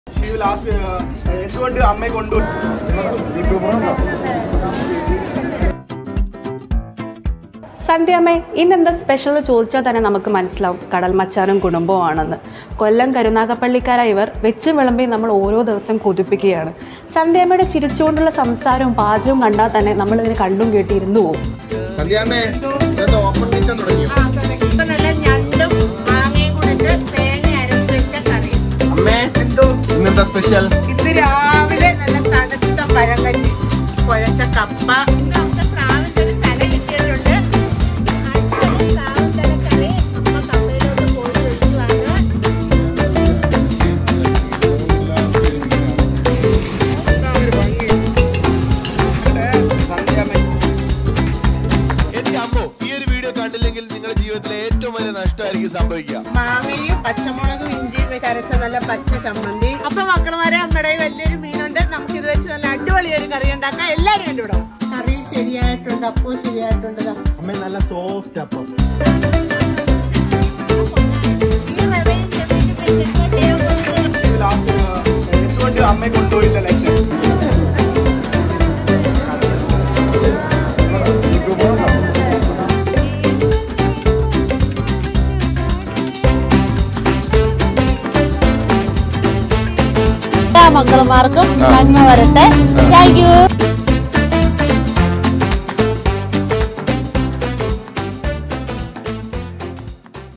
സന്ധ്യാമ്മ ഫുഡ് ഉണ്ടാക്കുന്നത് കണ്ടാൽ മതി... വയറ് നിറയും... | Kadal Machan | Insticts
സന്ധ്യാമ്മ ഫുഡ് ഉണ്ടാക്കുന്നത് കണ്ടാൽ മതി... വയറ് നിറയും... | Kadal Machan

കൊല്ലം കരുനാഗപ്പള്ളിക്കാരായ കടൽമച്ചാനും കുടുംബവും വെച്ചും വിളമ്പിയും നമ്മളെ കൊതിപ്പിക്കുകയാണ് .ചിരിച്ചു കൊണ്ടുള്ള ഇവരുടെ സംസാരവും പാചകവും സോഷ്യൽ മീഡിയയിൽ വൈറൽ ആണ് . kadalmachansandhyamma.

Genre: News
Language: Malayalam